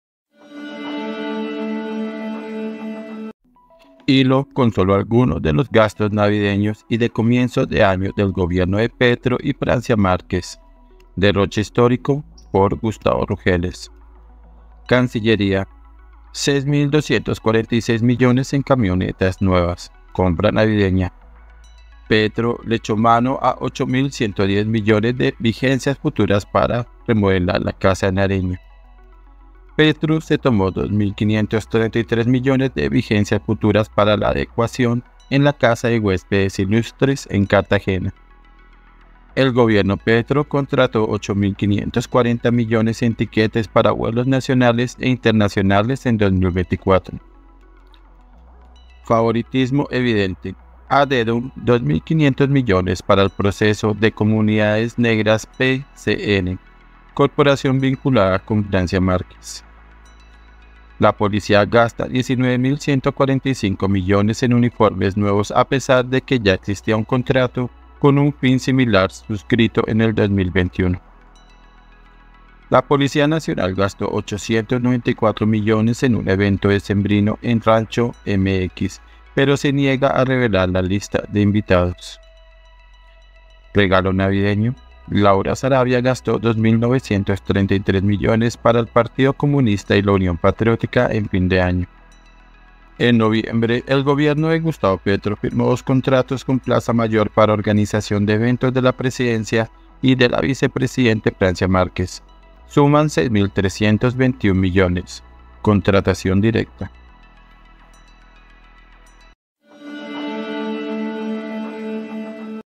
¿DERROCHE HISTÓRICO? ❌ Gastos navideños y de comienzo de año del gobierno de Petro
Hilo con solo algunos de los gastos navideños y de comienzo de año del gobierno de Petro y Francia Márquez ¿Derroche histórico? Datos
GUSTAVO RUGELES